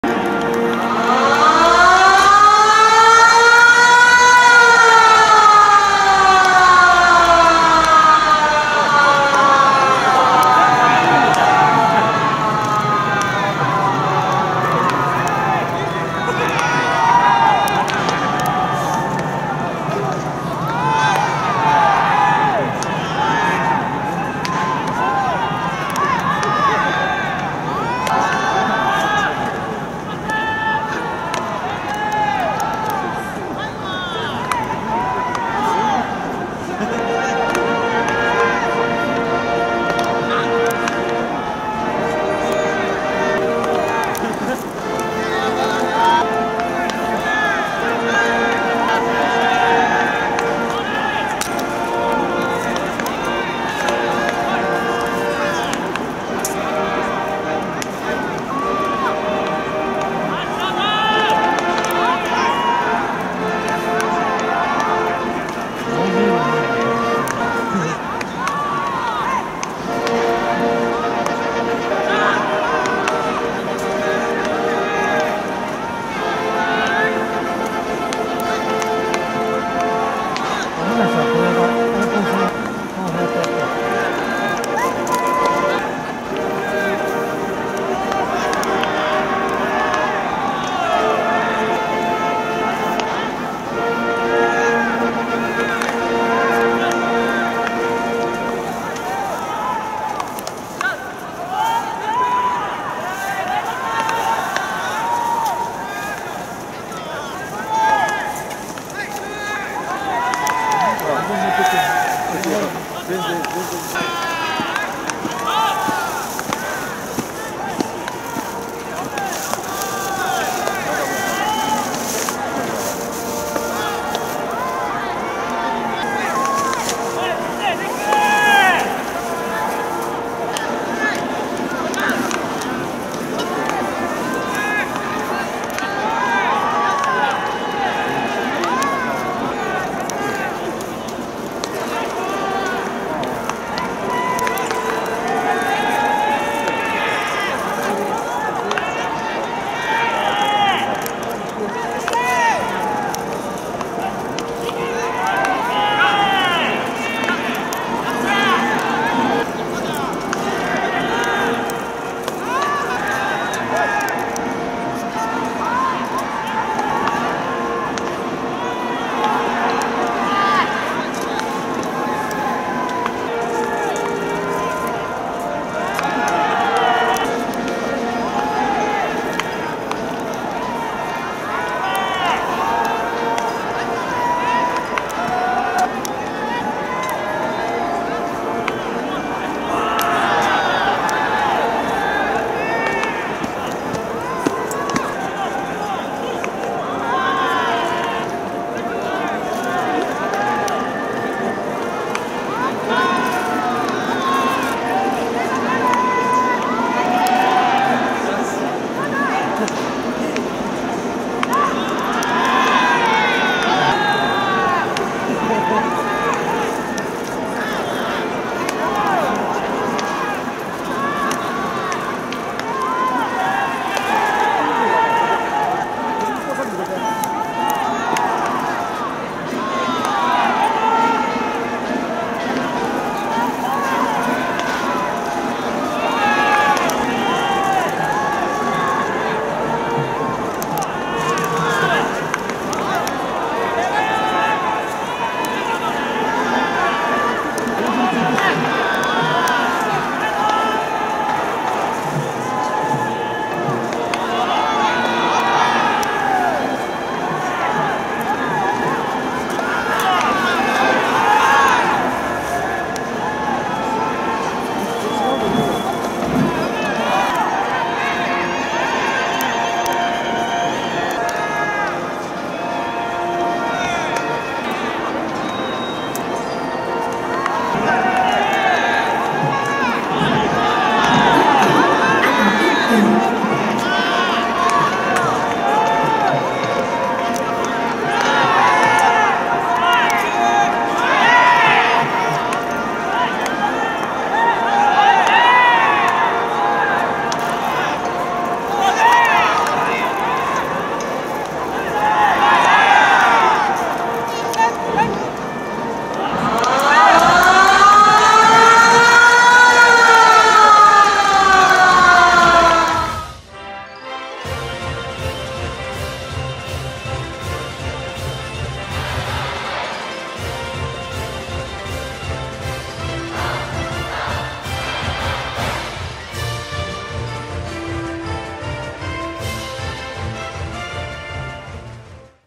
秋季大会で昨年の選抜覇者 東海大相模に4-1で勝利！優勝を狙う木更津総合 シートノック [第94回選抜高校野球]
木更津総合 試合前のシートノック

球場：
甲子園球場

※選手への誹謗中傷、過度な批判などのコメントは削除させてもらいます。